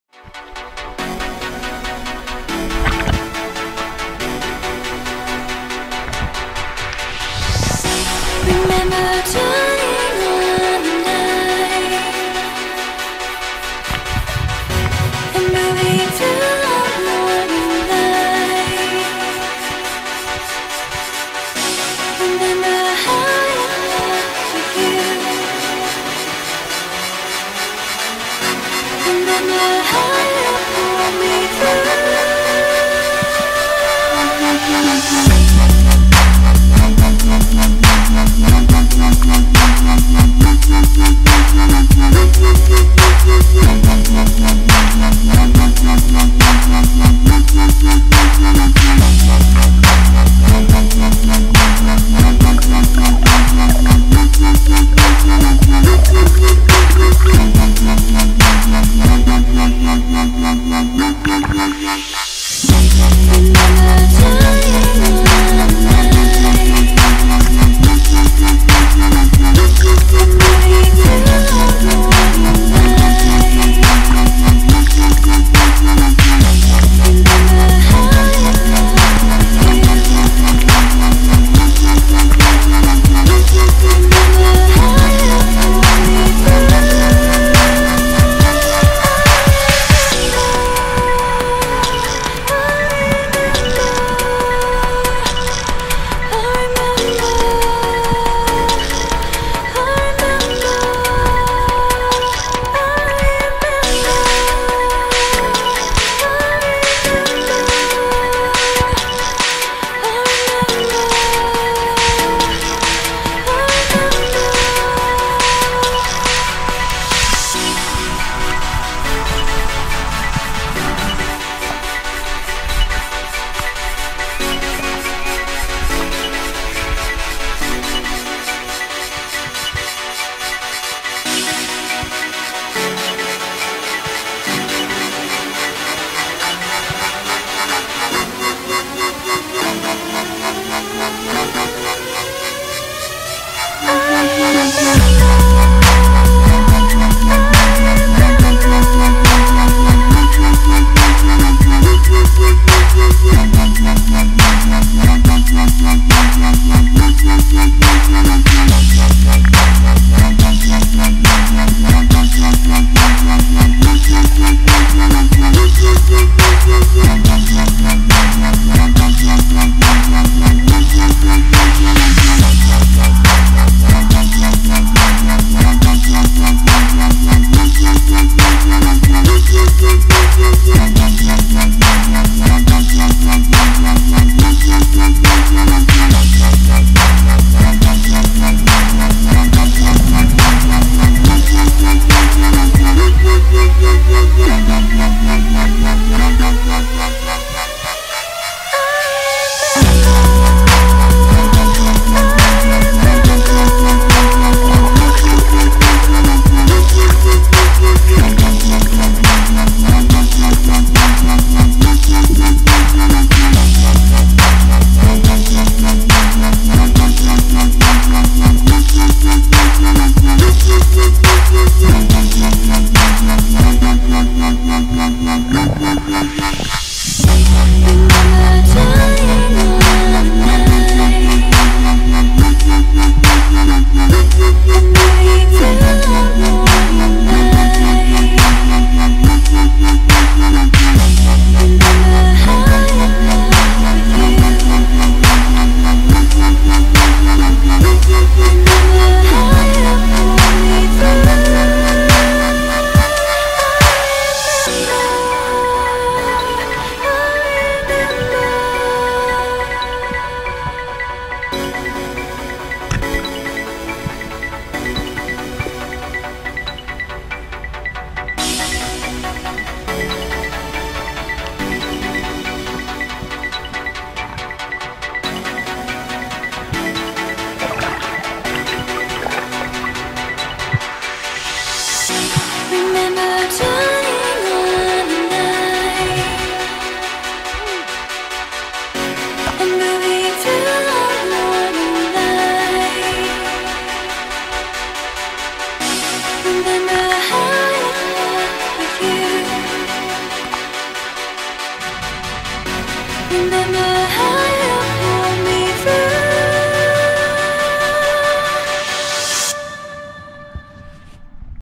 69 Subscriber Special: Getting Crabs and Spearfishing for Flounder
When the water gets hot, flounders start coming up to shallow waters. One of my favorite fish to hunt, because they blend in to their environment so well. Dives ranged between 10-40ft. The thermocline was around 30ft and that is why the visibility gets a lot better as I get deeper.